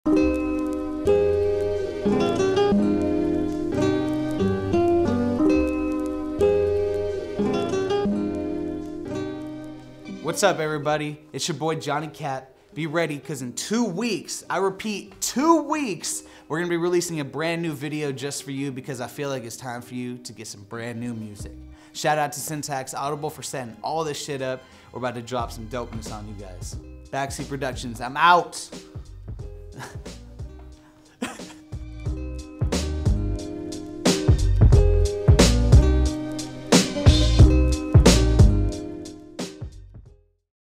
Thoughts of the Third Eye PROMO #1
Promo for a new video featuring Johnny Kap. Filmed and recorded by Syntax Audible